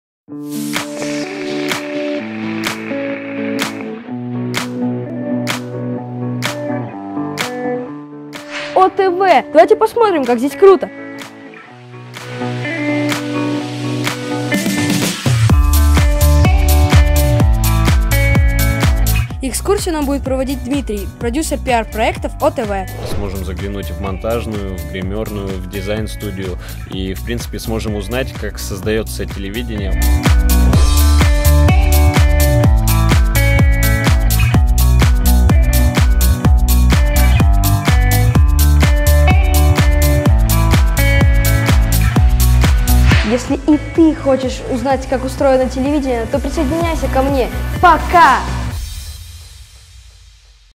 Экскурсия по медиахолдингу «ОТВ» глазами «ТелеШко»
ВЫ УЗНАЕТЕ МНОГО ИНТЕРЕСНОГО

Увидите привычные вещи с необычных ракурсов и побываете там, куда невозможно попасть не имея специального пропуска.

Познакомитесь с творческими профессиями и поймете нравится ли Вам эта работа.

Услышите о курьезных случаях, произошедших во время съемок и  оставшихся за кадром.

Увидите как снимается программа "Время новостей" и что делает ее ведущий за кадром во время прямого эфира.

Но самое главное, Вы окажитесь в роли ведущего программы "Наше утро", и почувствуете себя частичкой "фабрики грёз".

Ссылку прошу предоставить в комментариях.